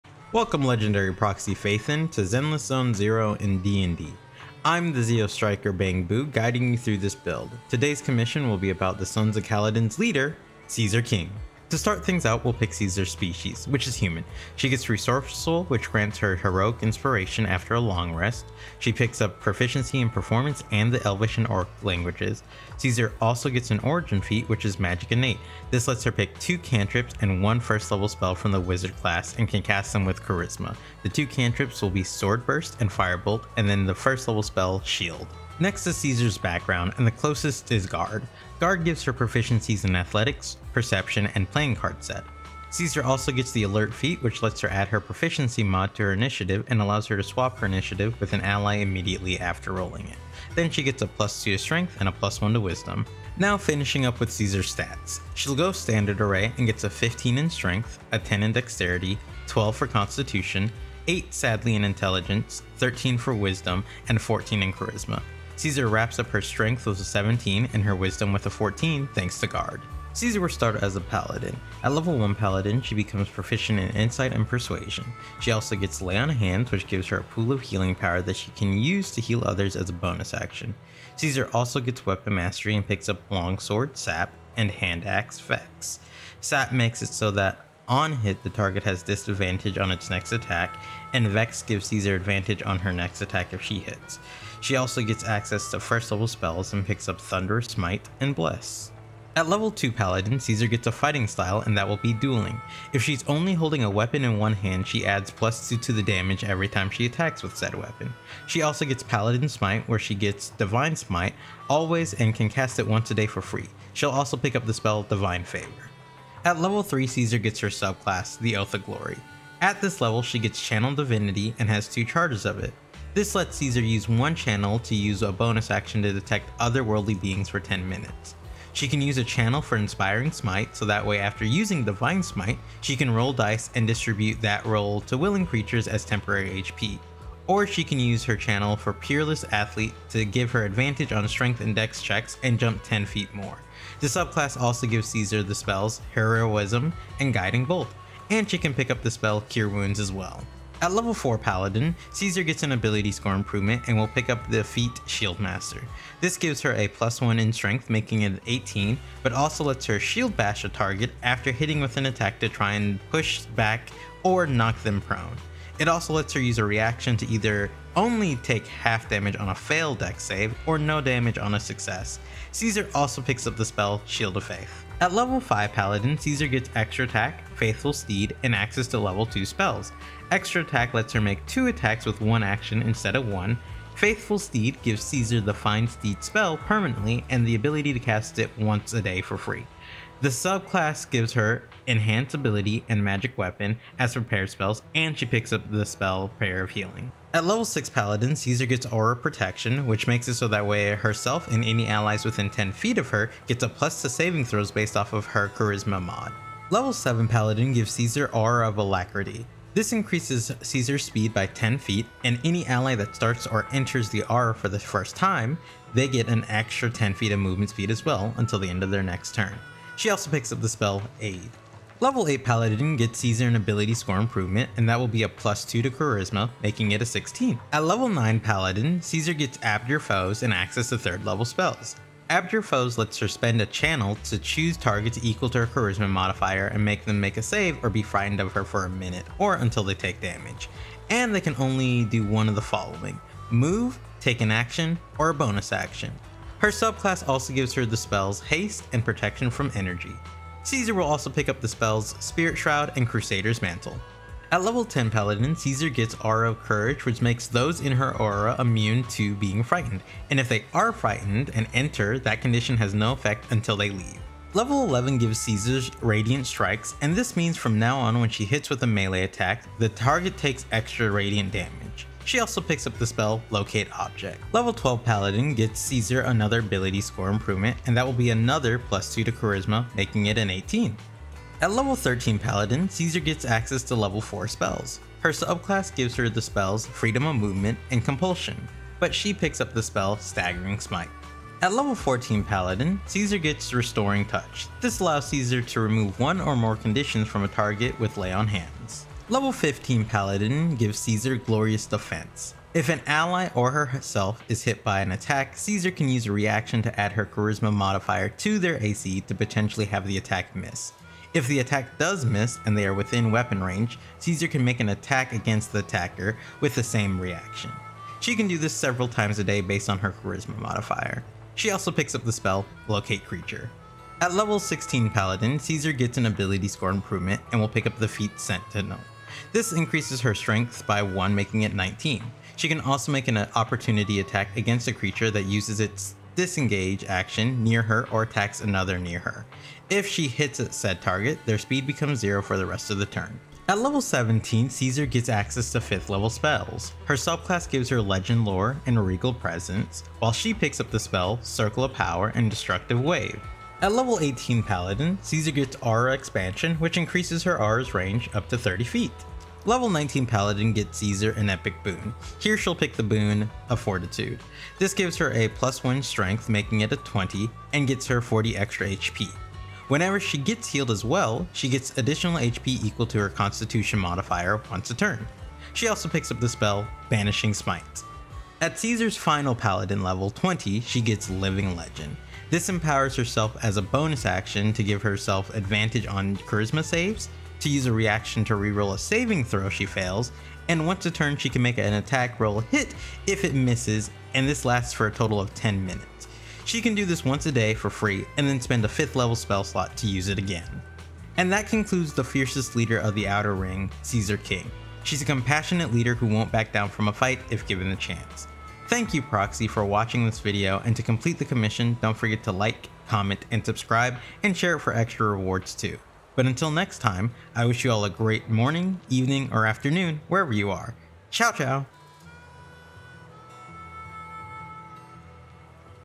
How to Build Caesar King in D&D (Zeneless Zone Zero in D&D)
Hello, Proxy, and welcome to Zenless Zone Zero in D&D 5E/2024! In today's commission, we'll build  Caesar King from levels 1-20.

▼ OPEN ME ▼
🕘TIMESTAMPS
0:00 Introduction
0:11 Species, Background, and Stats
1:09 Level 1 -5
3:17 Level 6 - 10
4:24 Level 11 - 15
5:24 Level 16 - 20
6:41 Ending
SOCIALS